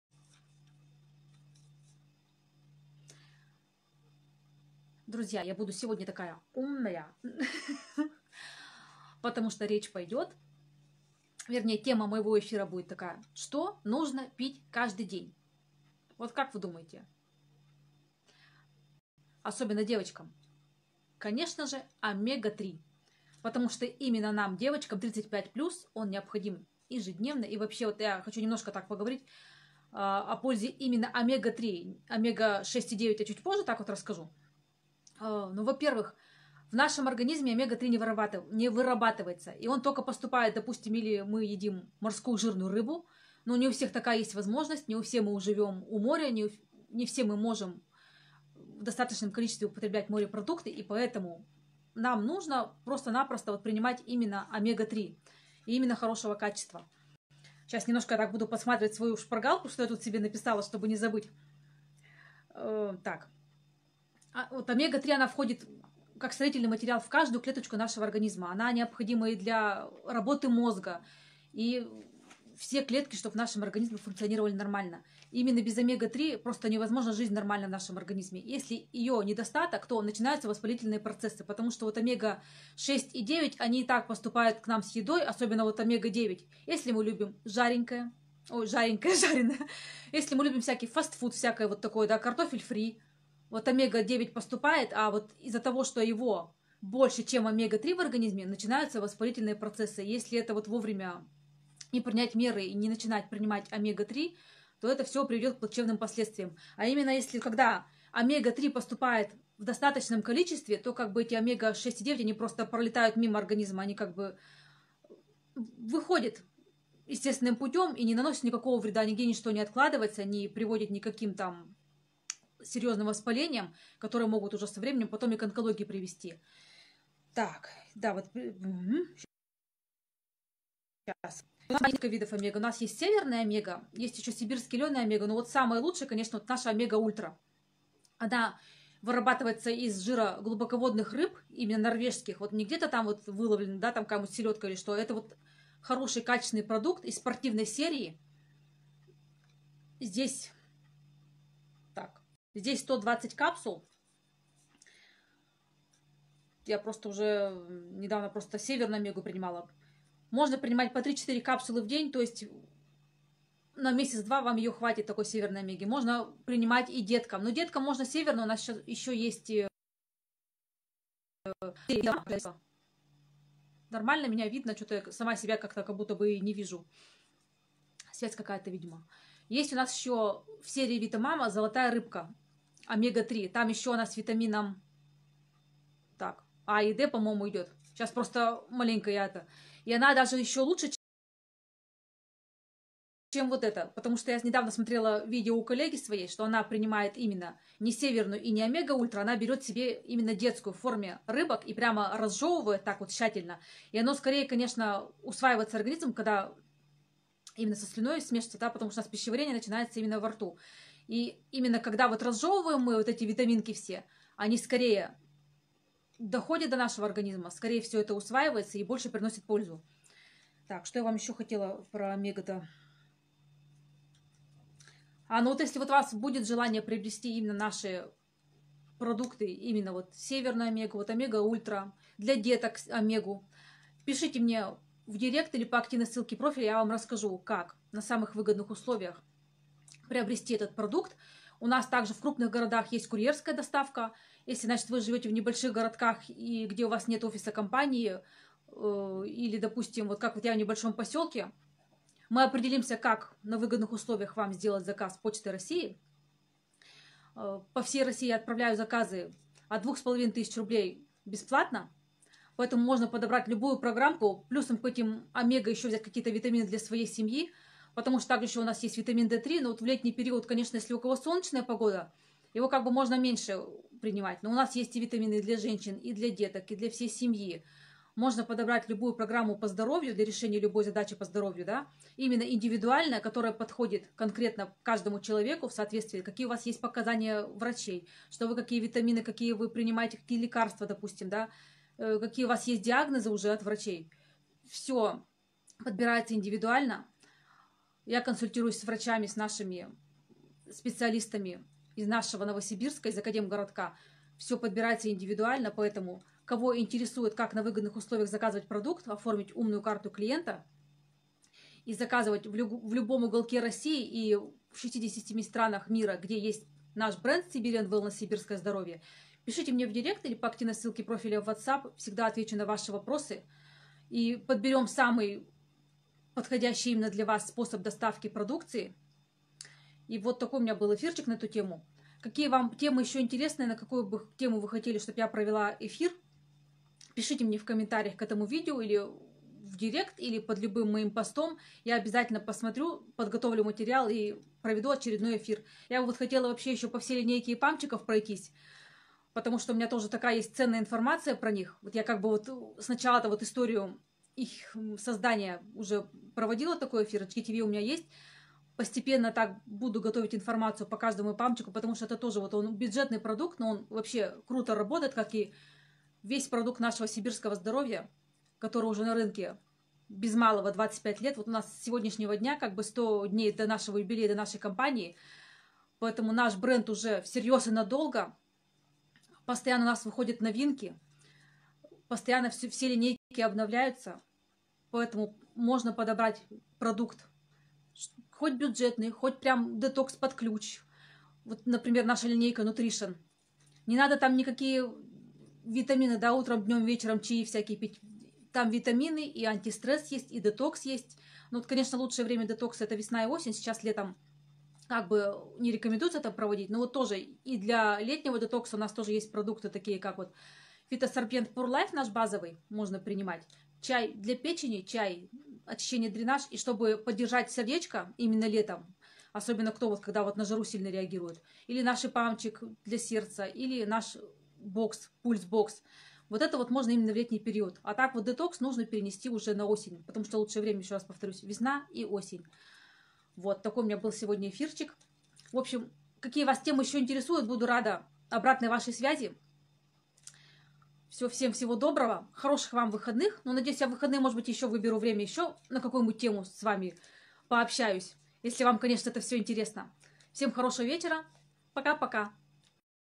Польза Омега-3.Омега 3 Сибирское здоровье. Сибирское здоровье. Siberian Wellness
Что нужно пить каждый день? Запись прямого эфира в Инстаграм.

ஜ СВЯЗАТЬСЯ СО МНОЙ В СОЦ.СЕТЯХ═══ஜ